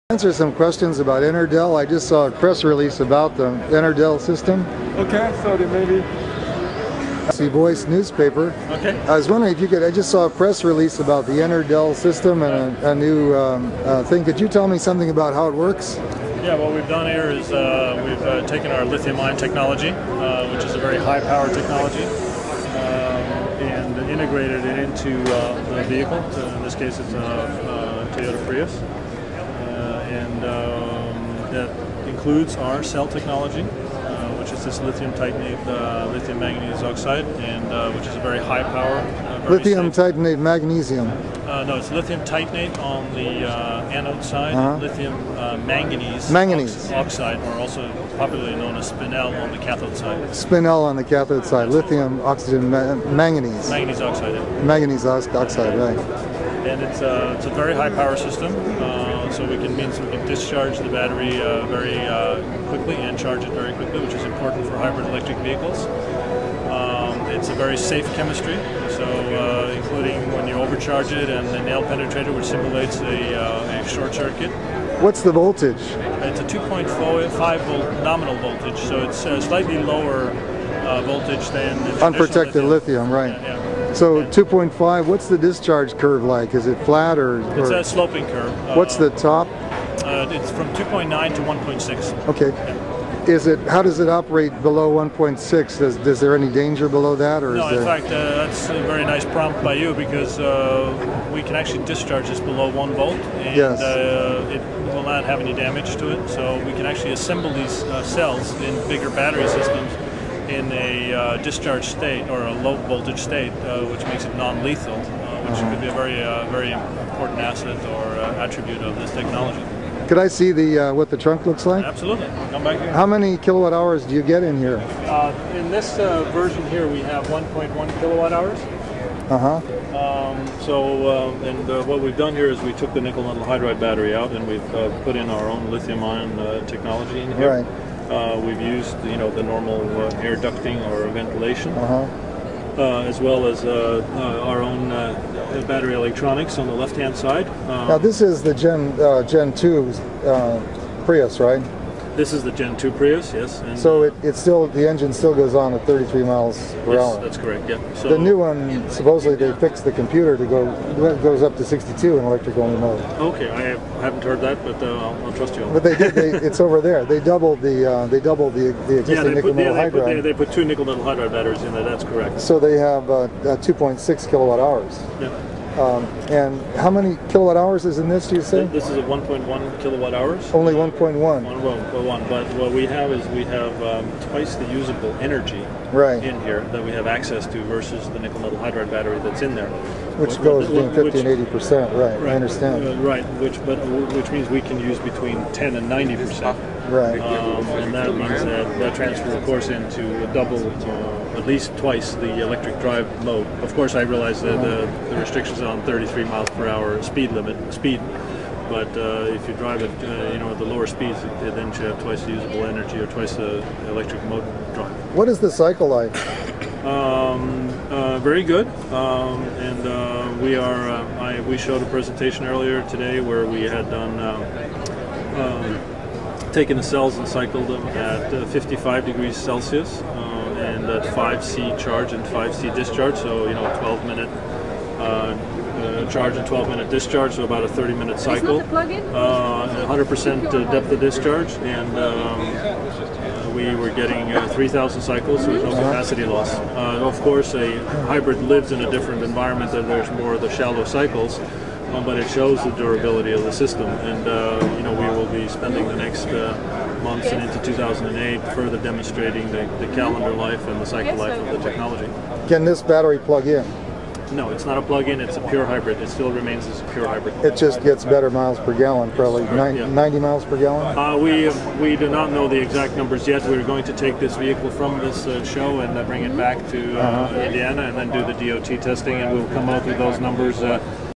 EVS23 Enerdel Lithium Prius - Tape 5
Enerdel removed the 1.2 kWh NiMH Prius battery and replaced it with a 1.1 kWh Lithium Titanate/Lithium MnO4 battery which is addressable for more energy than the Prius can get from its NiMH.

The interesting fact is that Enerdel seems to have thus penetrated the Prius computer source code.  If so, why not just keep the NiMH battery, just make it more addressable?